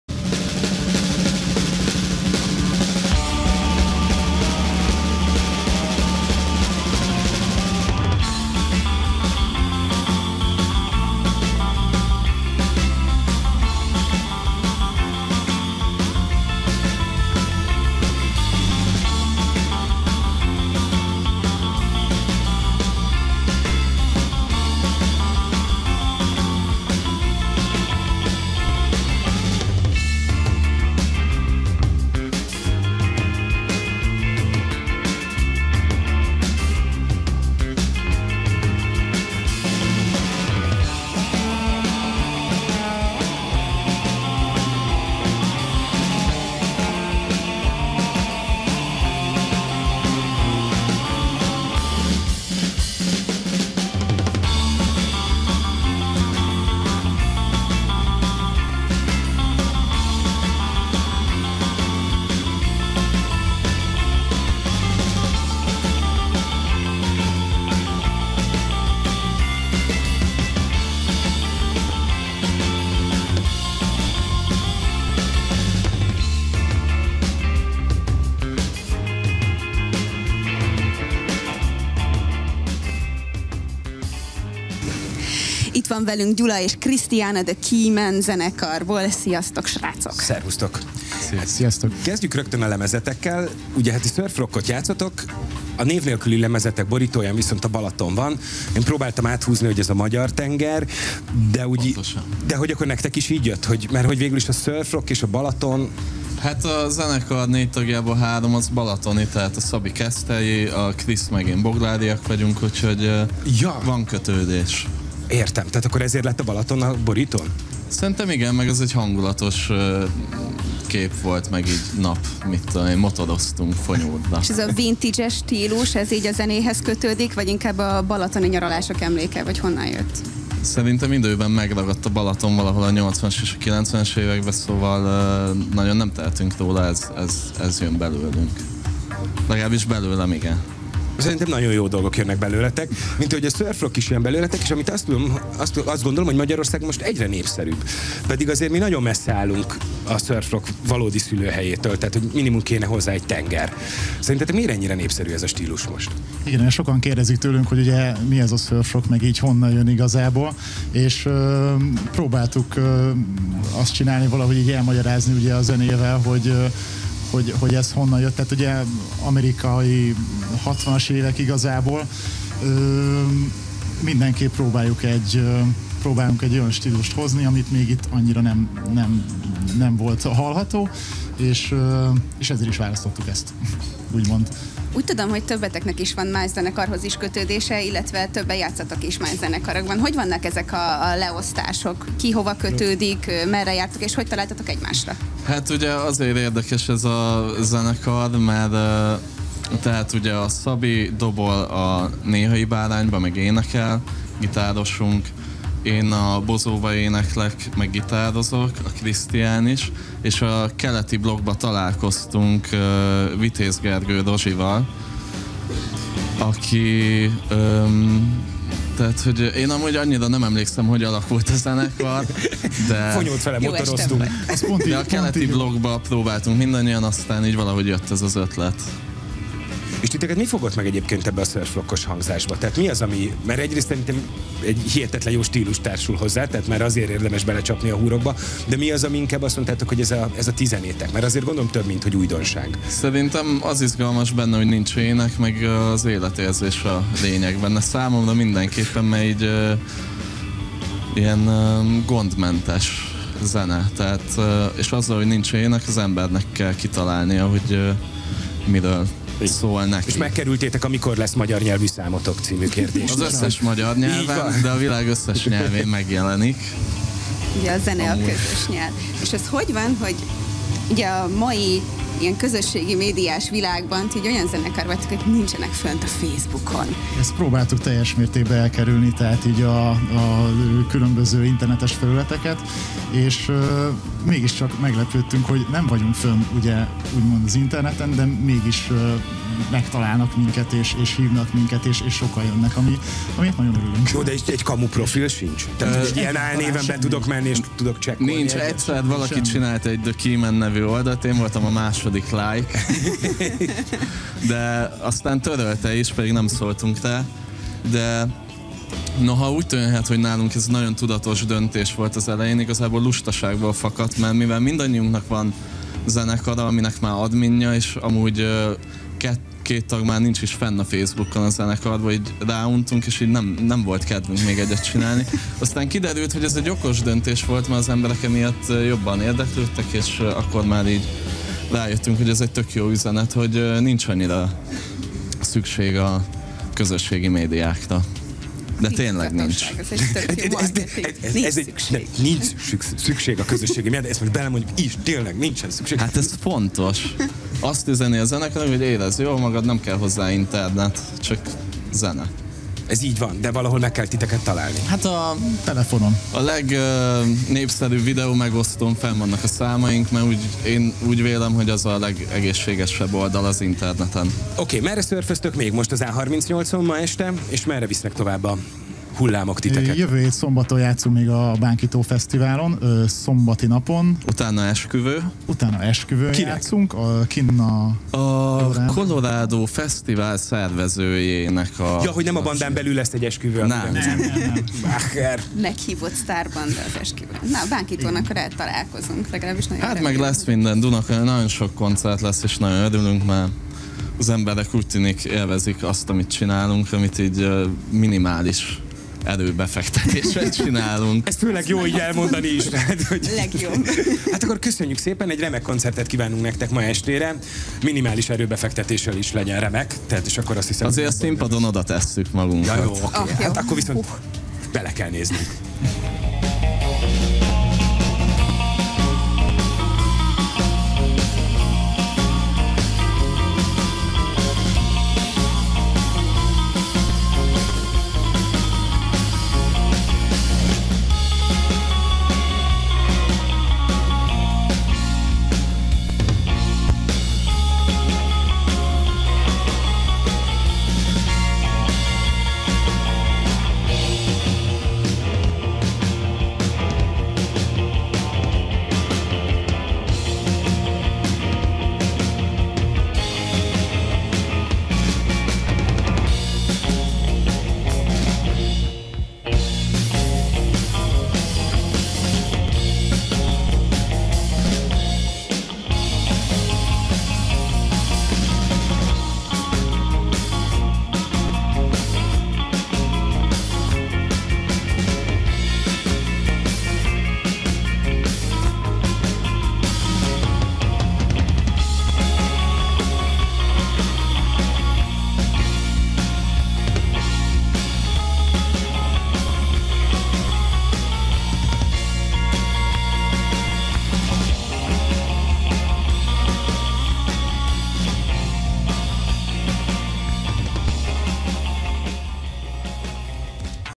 Keeymen az A38-on – koncert és interjú
Surf rock - a Balatonról. A zenekar három tagja balatoni: Szabi keszthelyi, Krisz és Gyula boglári. A lemezborító fotó egy vidám napon készült, mikor épp átmotoroztak Fonyódra. Teknyős Krisztián basszusgitáros és Szabó Gyula gitáros arról, miért jó a hatvanas évek amerikai beach feelingjét elhozni 2018 Magyarországára zenében. Nézz bele a koncertbe!